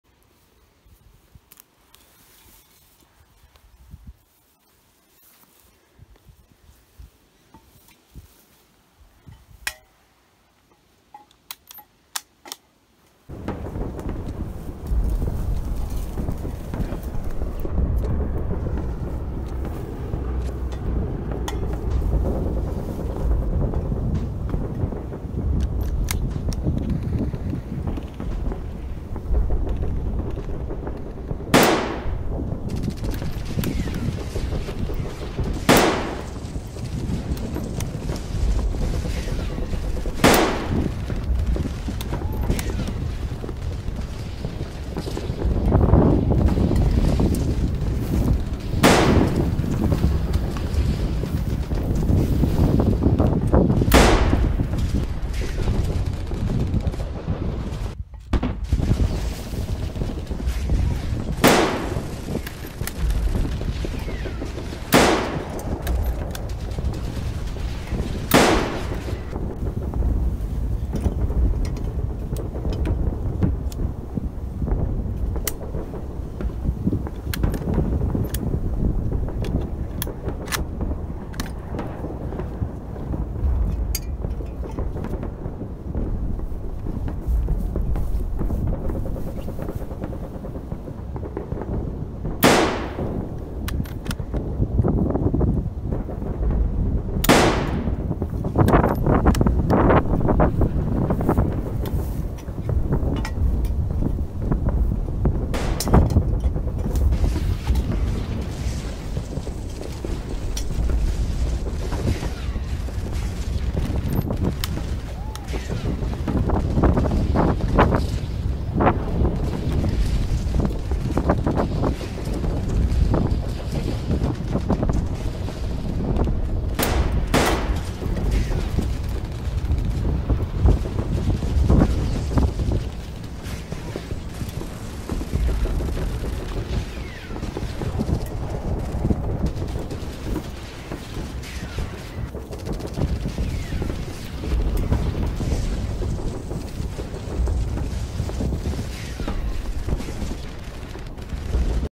Reconstitution résistance 39-45 partie 2
Je vous présente une reconstitution de la vie d'un résistant américain durant la la guerre 39-45.

Si vous voulez d'autre reconstitution de se genre dite le nous et nous feront tout pour le mettre en oeuvre.